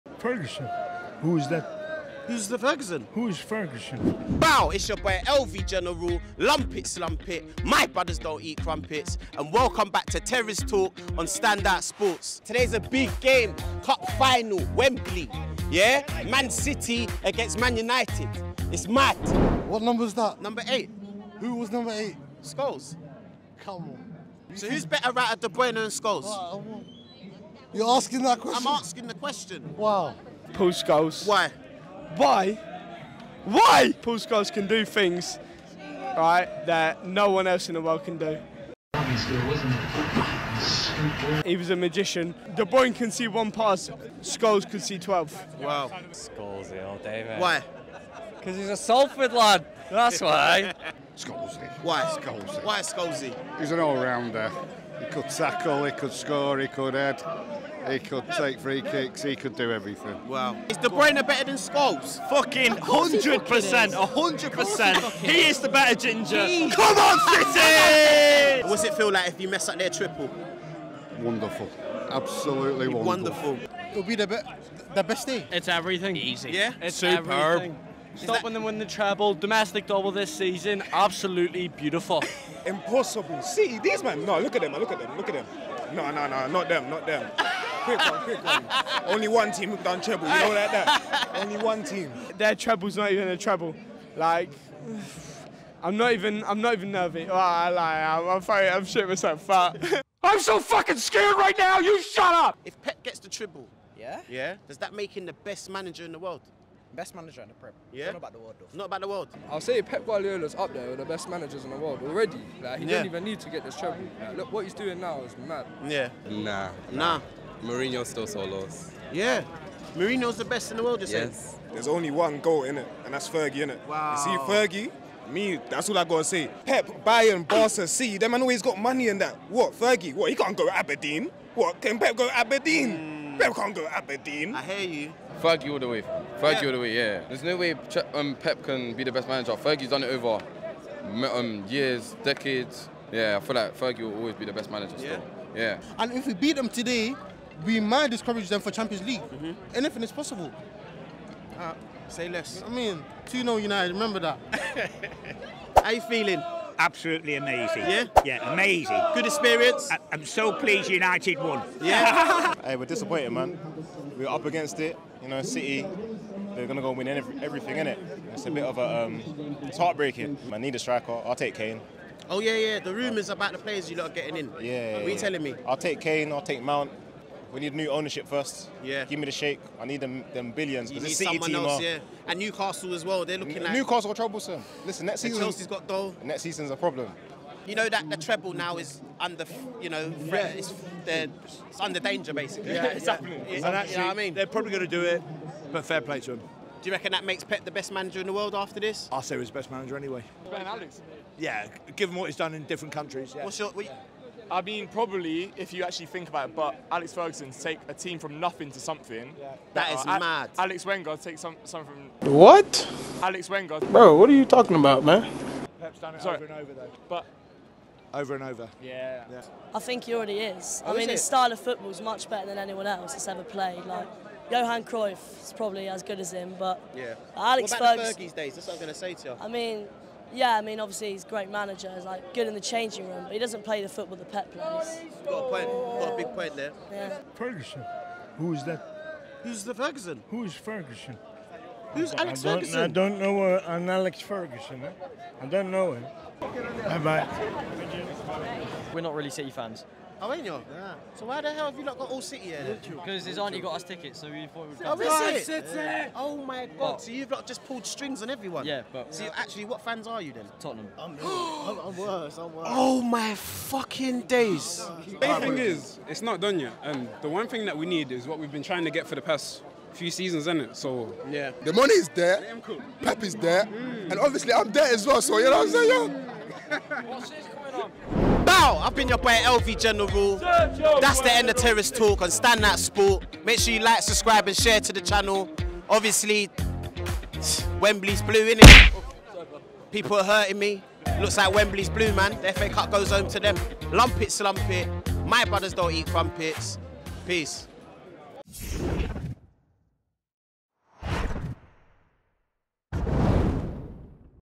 YOU THINK PEP CAN GO ABERDEEN?! || FA Cup Final - Terrace Talk by Stand Out Sport
This week we were at Wembley for the Final Terrace Talk of the season from the FA Cup Final & it didn't disappoint. City were hunting the Treble & in the end; there was NOTHING United could do about it!

Hear from both sets of fans as Manchester City moved ONE STEP CLOSER to the Treble!